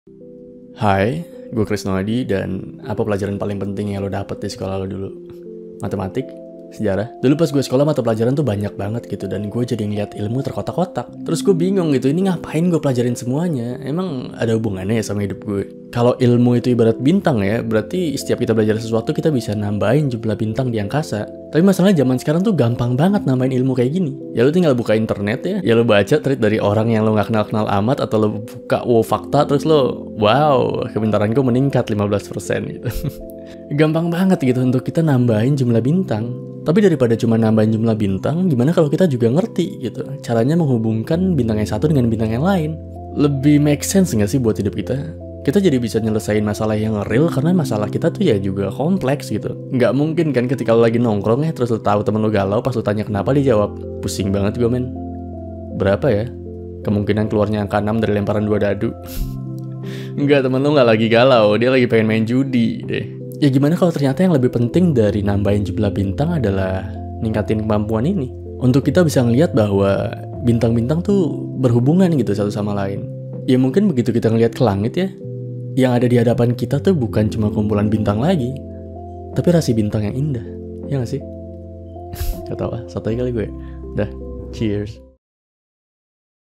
Setelah Gede, Ternyata Pendidikan, tuh... | In My Sotoy Opinion
Kalian suka mikir enggak, sebenarnya ilmu yang kita pelajari di sekolah, tuh, penting enggak, sih? Hubungannya sama hidup kita tuh apa?

(Narasi)